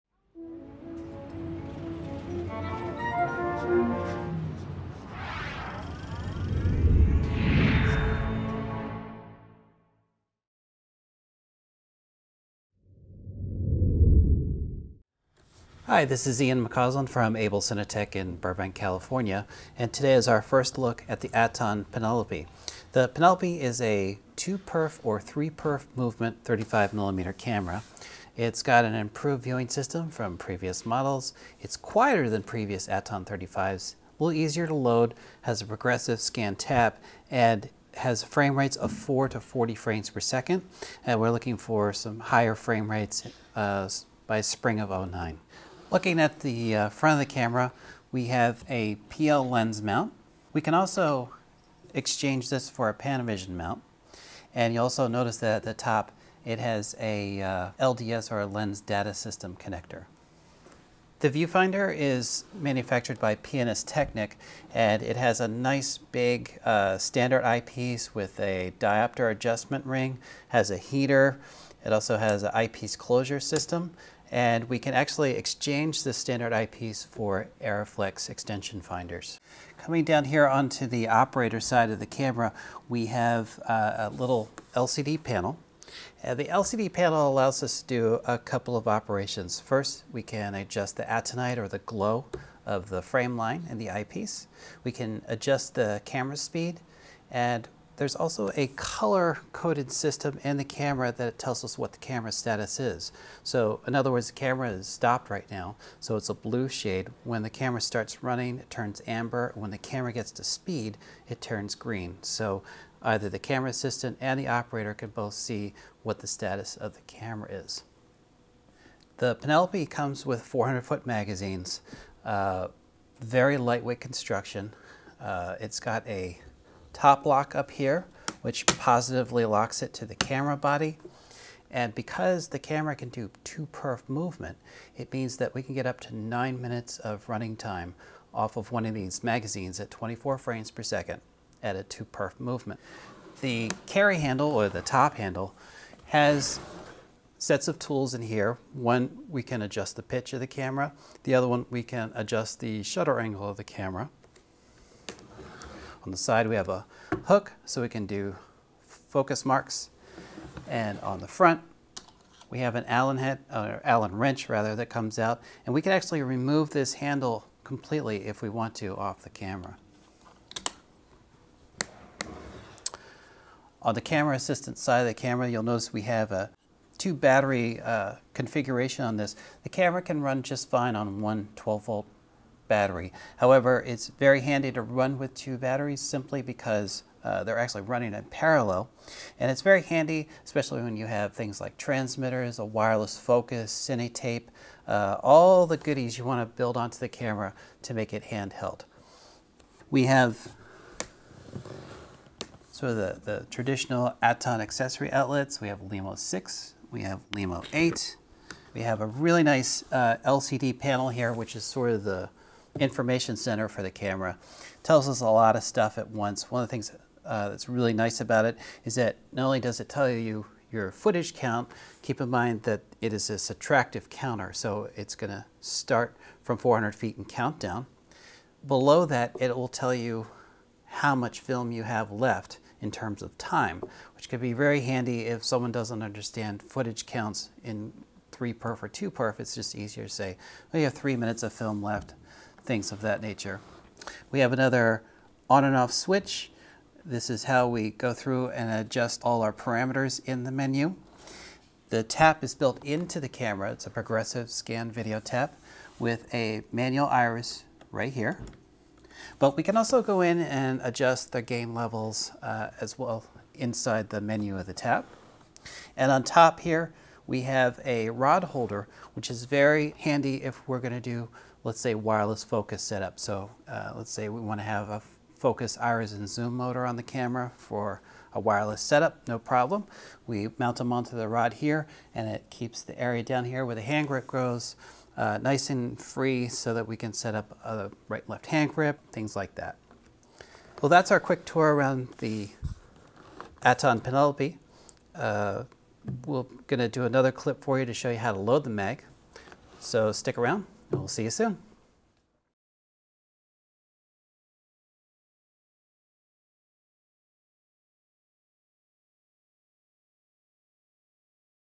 Penelope Overview
In this video, Ian McCausland, Rental Equipment Manager for Abel LA, takes you on a tour of the new Aaton Penelope 35mm camera. The Penelope is convertible between a 2-perf or 3-perf movement, quieter than previous Aaton 35s, and offers a new viewing system, as well as other enhancements.